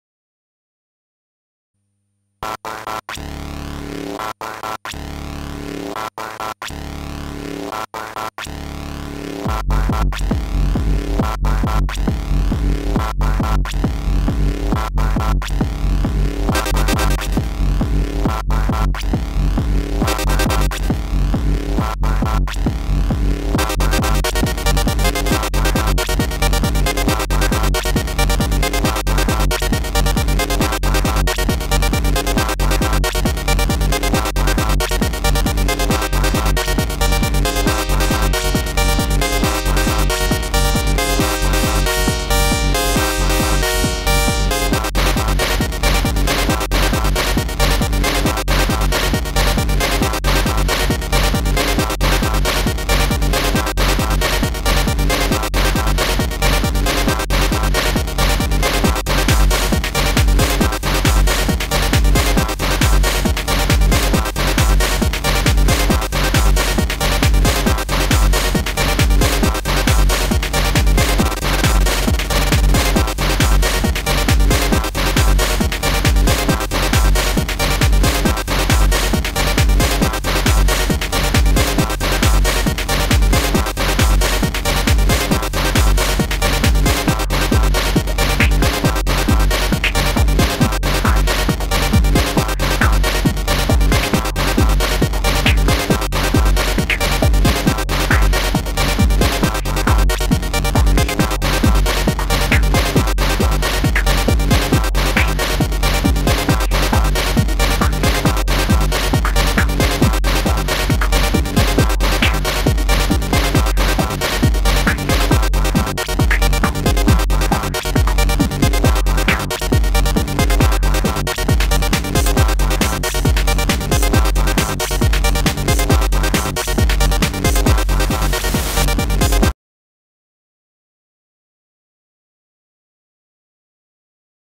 Jamuary2021 Day 2
Usually I jam on different setup every day for the Jamuary , but this time I left it as it is. Originally it used to be a two part jam, but turned out that sync didn't start properly and I discovered it only after transition to another kick drum pattern. So this is hard cut from the first half. Sorry :D

Jamuary is a music challenge where you post a video of you jammin' every day for the month of January. I find out about this idea at cuckoomusic channel back in 2017

Browse the hashtag Jamuary2021 and get inspired. Get to know musicians you had never heard about, and perhaps even make some friends along the way.